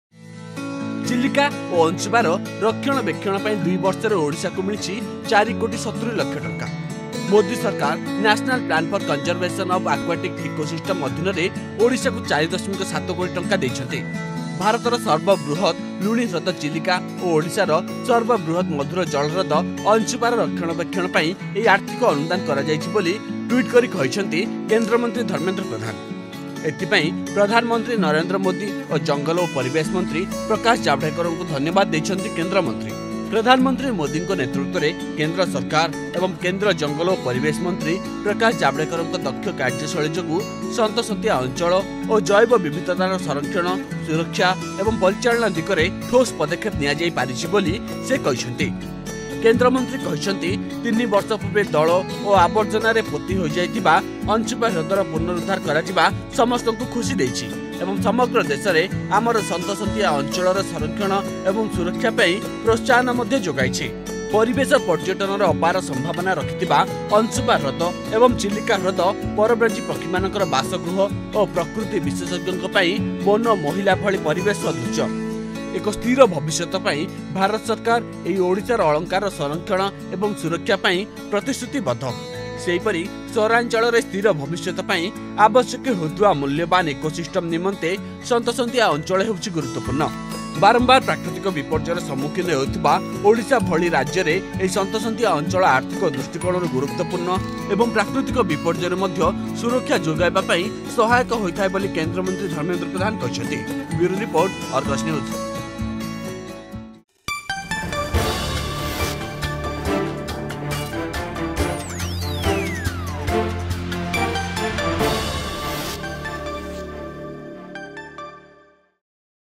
ଚିଲିକା ଓ ଅଂଶୁପାର ରକ୍ଷଣାବେକ୍ଷଣ ପାଇଁ 2ବର୍ଷରେ ଓଡ଼ିଶାକୁ ମିଳିଛି 4.7 କୋଟି
ଚିଲିକା ଓ ଅଂଶୁପାର ରକ୍ଷଣାବେକ୍ଷଣ ପାଇଁ 2ବର୍ଷରେ ଓଡ଼ିଶାକୁ ମିଳିଛି 4.7 କୋଟି । କେନ୍ଦ୍ରର ଅନୁଦାନ ଯୋଗୁ ଆଜି ଅଂଶୁପା ହ୍ରଦର ପୁନରୁଦ୍ଧାର କରାଯାଇଛି । ଏହି ଦୁଇଟି ହ୍ରଦ ପରିବାଣ ପର୍ଯ୍ୟଟନର ଅପାର ସମ୍ଭାବନା ରଖିଛି । ତେଣୁ ଏନେଇ ପ୍ରଧାନମନ୍ତ୍ରୀ ନରେନ୍ଦ୍ର ମୋଦି ଓ କେନ୍ଦ୍ରମନ୍ତ୍ରୀ ପ୍ରକାଶ ଜାଭେଦକରଙ୍କୁ ଧନ୍ୟବାଦ ଜଣାଇଛନ୍ତି କେନ୍ଦ୍ରମନ୍ତ୍ରୀ ଧର୍ମେନ୍ଦ୍ର ପ୍ରଧାନ